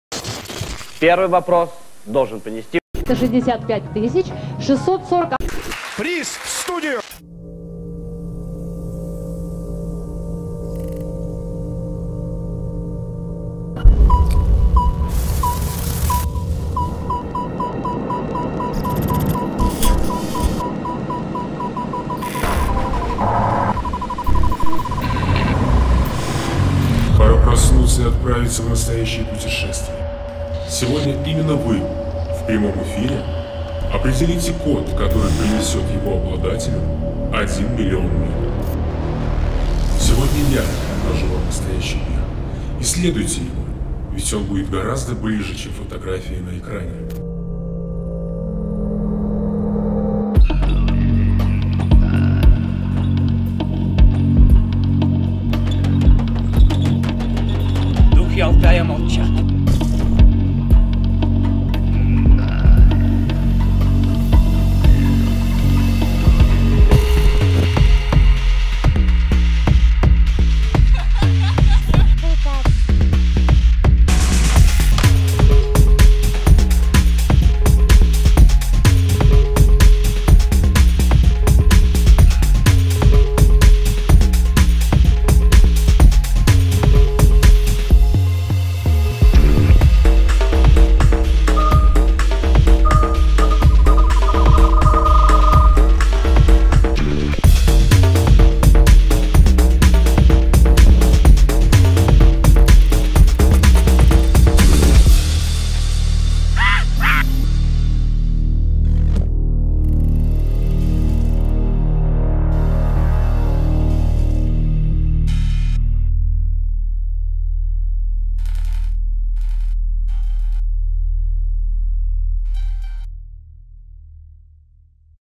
S7 Airlines / Night Flight Show vol.2
Inspired by S7 Airlines advertising campaign, we created and proposed a new format for the event. Experience Fiction is a fictional story built on the audience experience, where event guests participate in a random choice of a number and people watch it online.

A simulation of a night flight was created at the hotel: remote and interesting cities were selected from the S7 Airlines route network. Hotel rooms have been transfromed into miniature copies of Bangkok, Berlin, Tokyo, Gorno-Altaysk, and Barcelona.

The guests of the Night Flight Show were divided into crews to go on an exciting journey to different countries. In each country a number was selected and ID code of 6 digits was formed. Based on IDs, there was a winner chosen, receiving 1,000,000 miles that can be spent on a trip around the world.